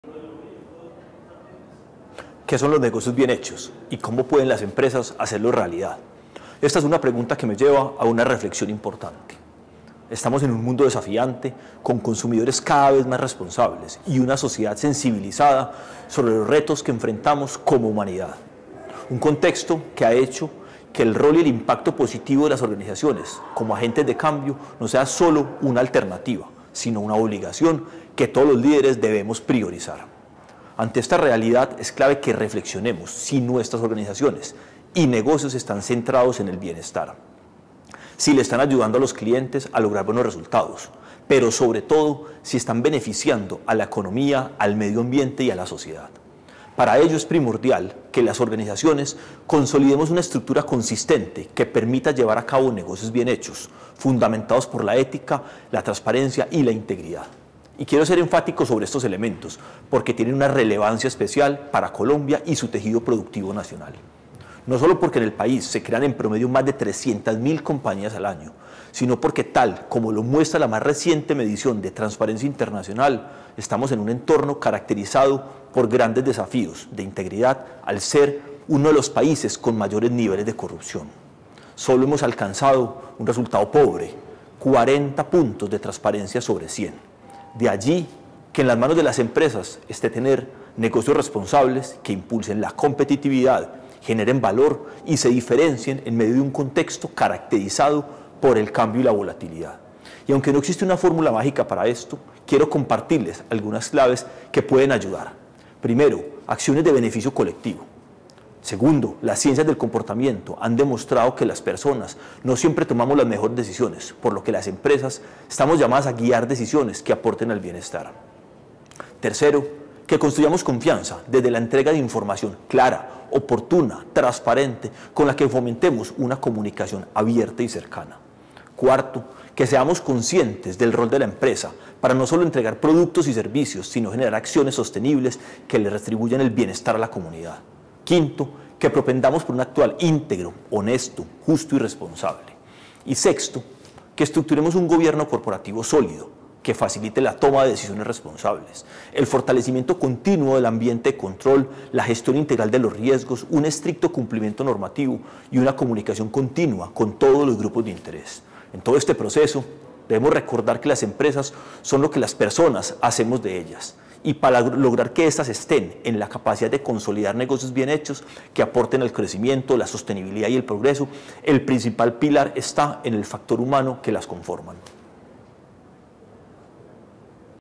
Videocolumna de Julián Mora Gómez, vicepresidente corporativo del Grupo Bancolombia
¿Qué son los negocios bien hechos y cómo pueden las empresas hacerlos realidad? En este espacio el vicepresidente corporativo del Grupo Bancolombia aborda conceptos y estrategias para negocios bien hechos en el país